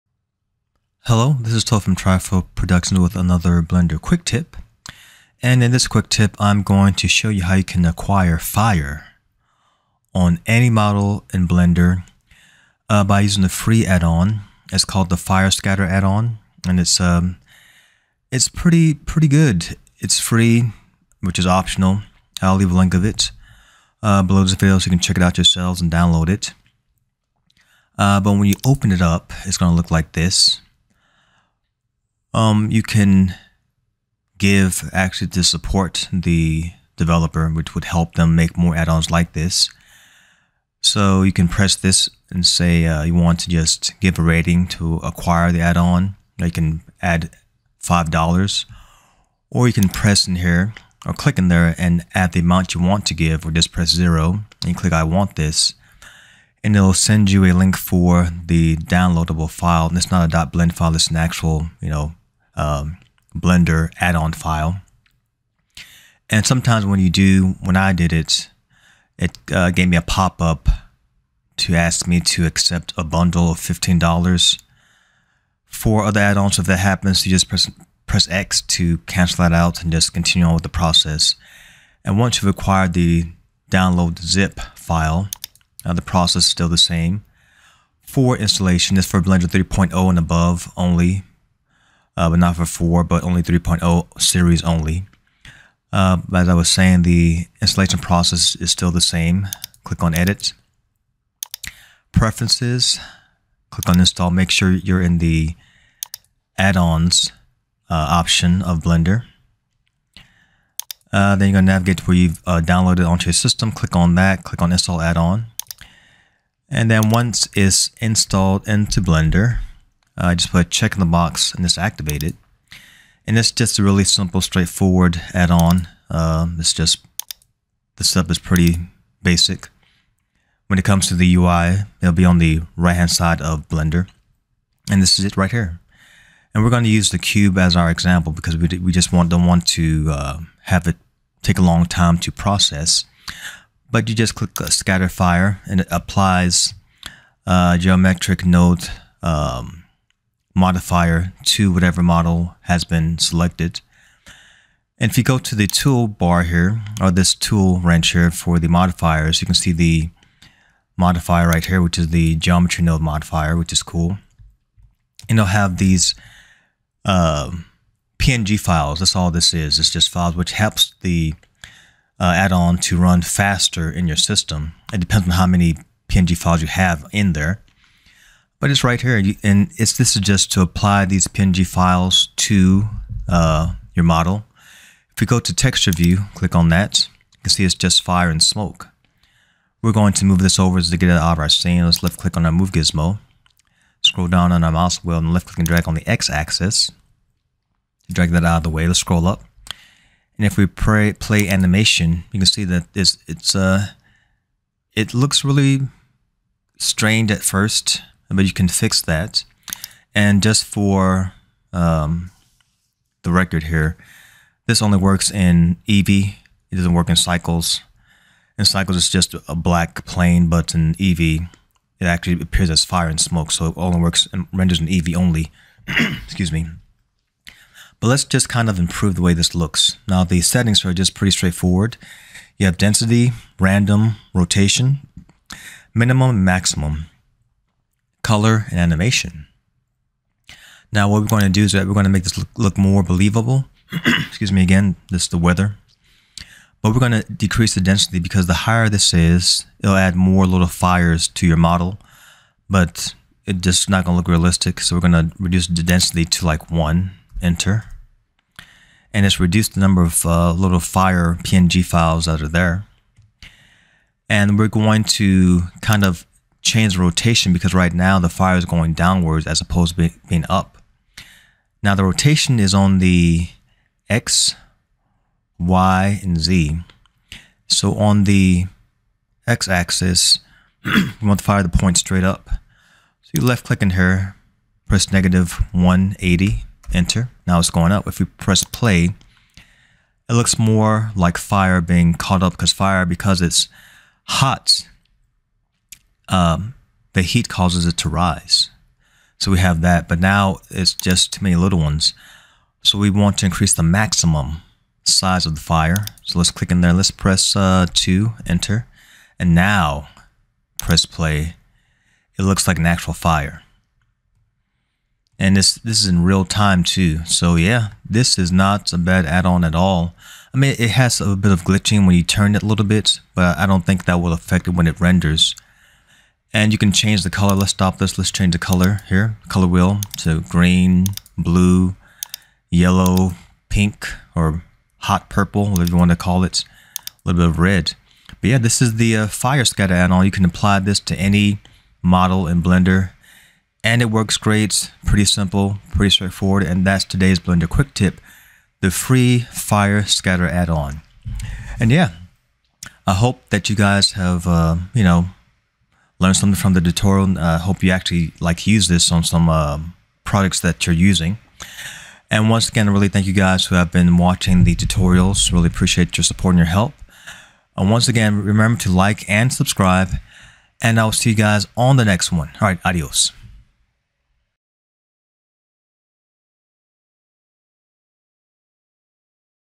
Blender Quick Tip: The Free Fire Scatter addon
In this tutorial, you'll learn how to add fire to any model in blender by using the free and easy to use fire scatter addon.